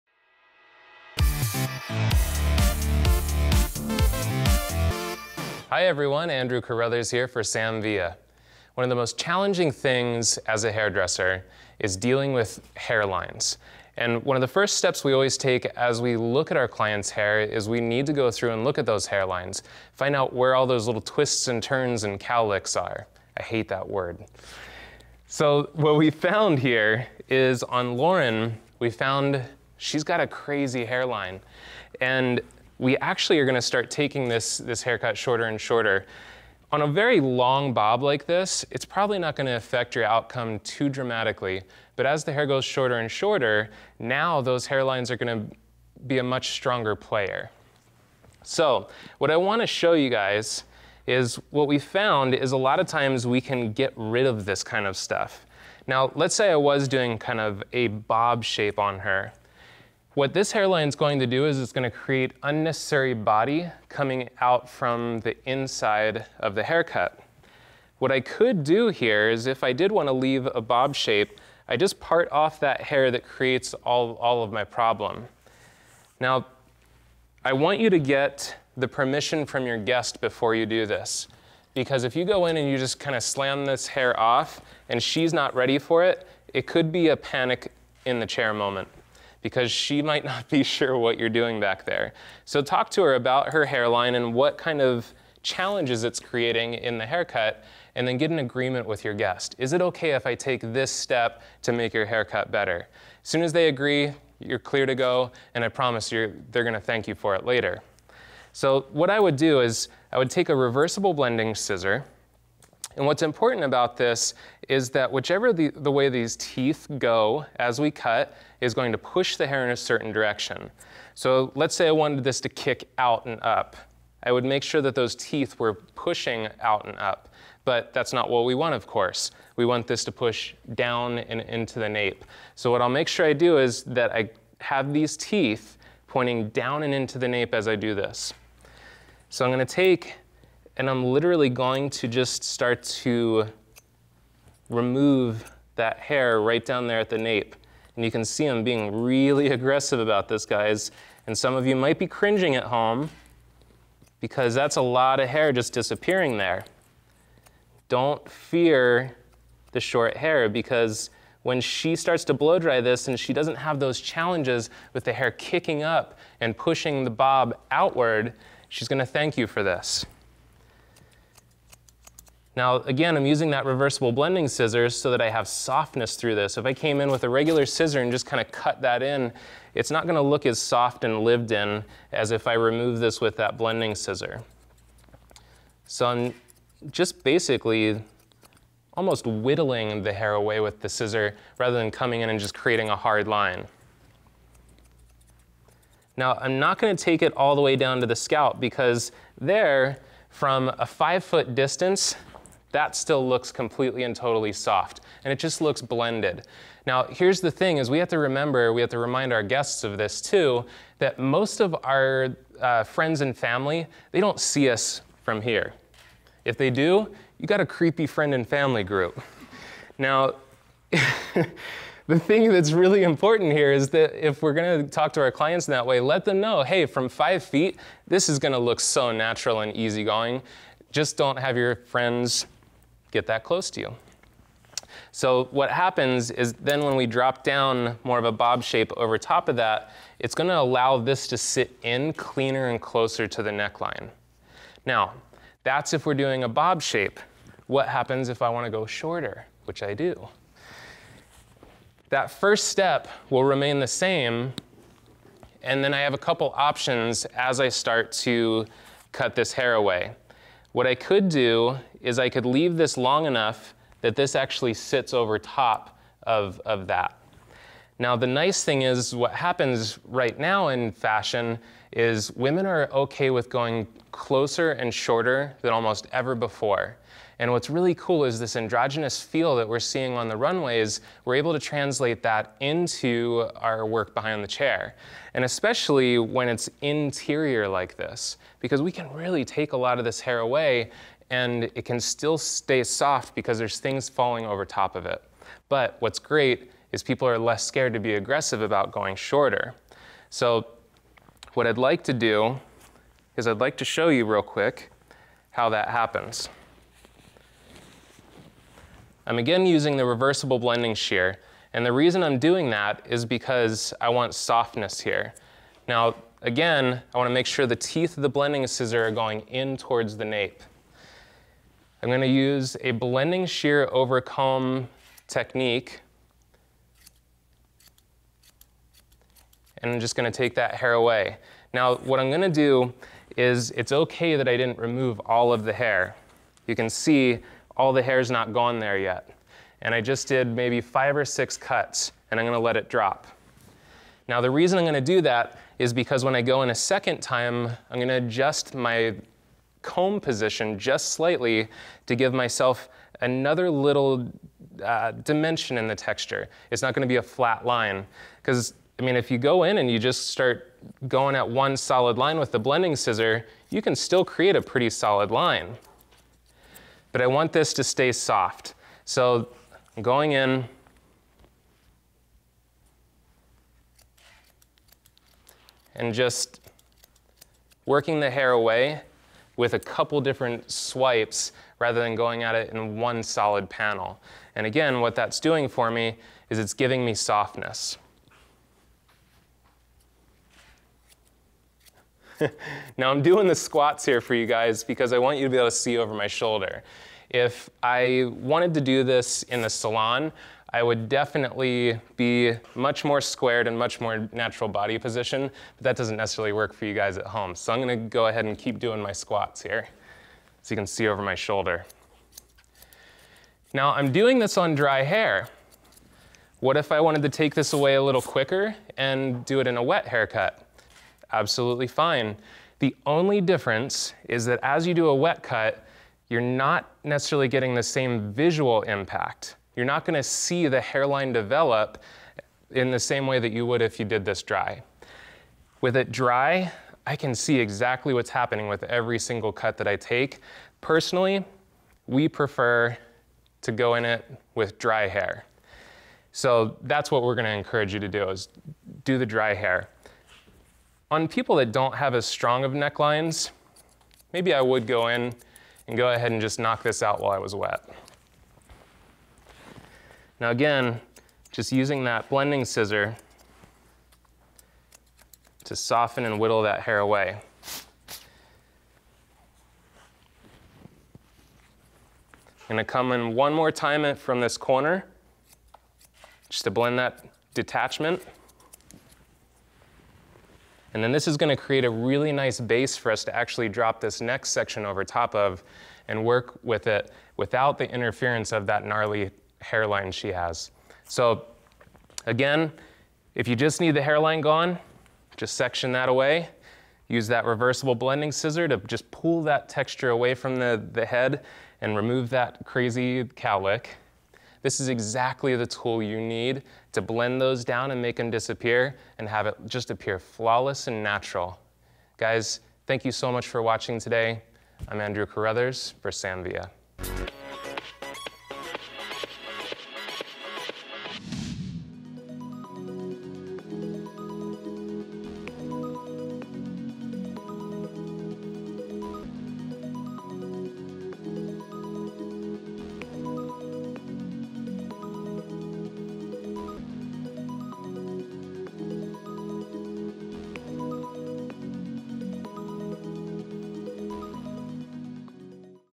Undercut Technique To Collapse The Shape Of Your Bob Haircuts: Cutting Difficult Hairlines
Nape hairline interference! You know that hair is going to pop up or straight out from the head the minute you cut it, right? Wrong! Not if you know how to manage it! In this video Andrew Carruthers demonstrates how to collapse the neckline with an undercutting technique that will make erratic hairline growth easy to manage!

Get Social with Sam Villa